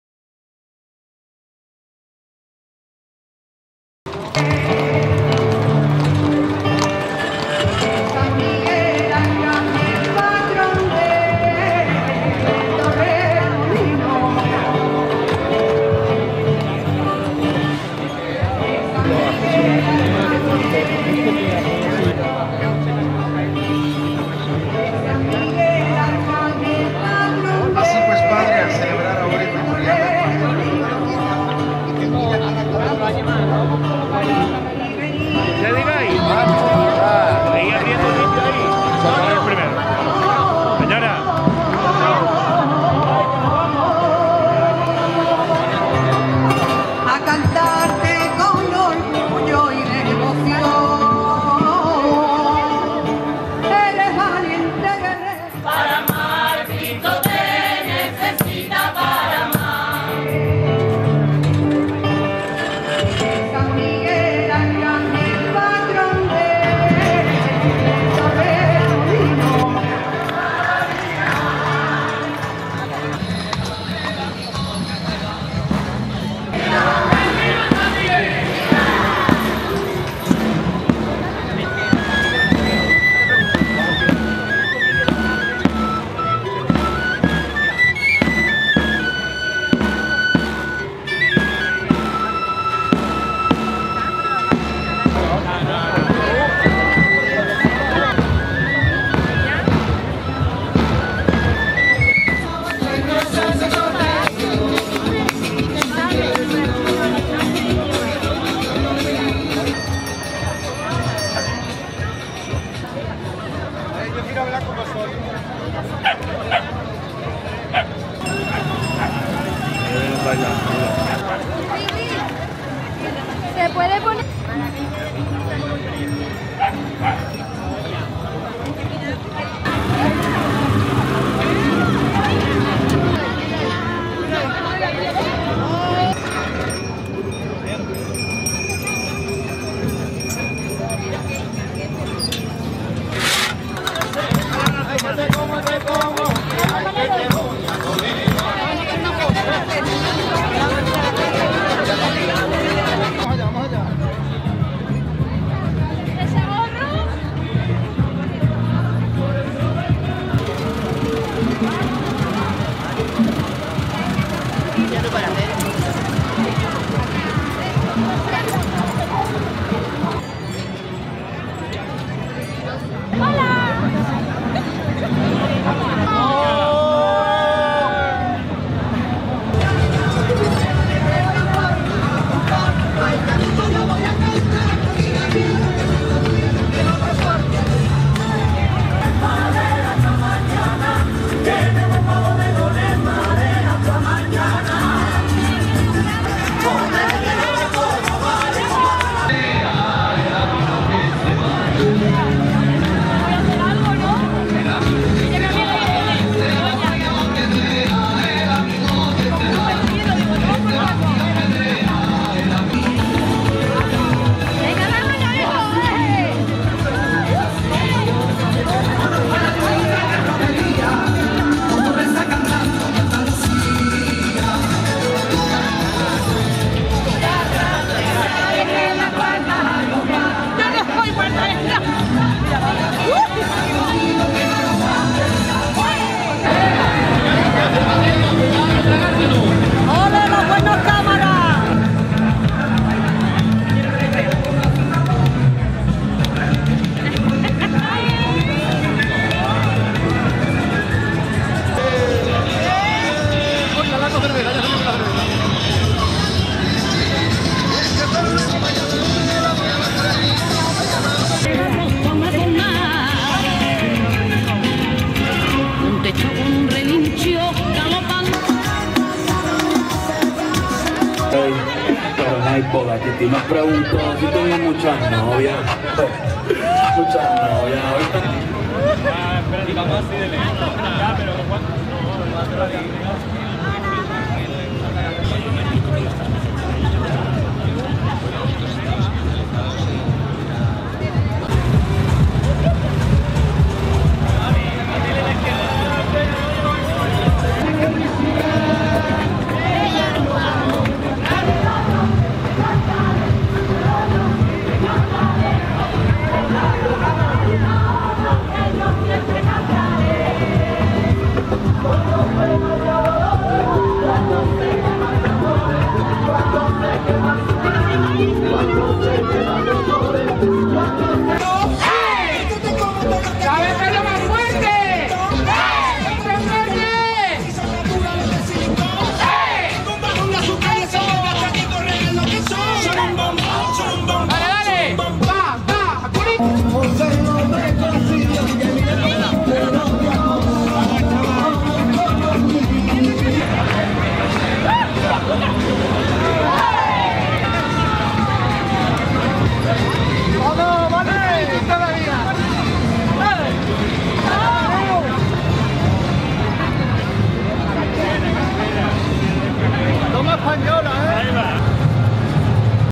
Romeria de San Miguel, Torremolinos 25/09/2022 (VIDEO 1 DE 3)
El pasado 25 de septiembre de 2022, se celebró en la localidad de Torremolinos (Málaga) la romería en honor de San Miguel.
Este es el primer vídeo de tres que hay.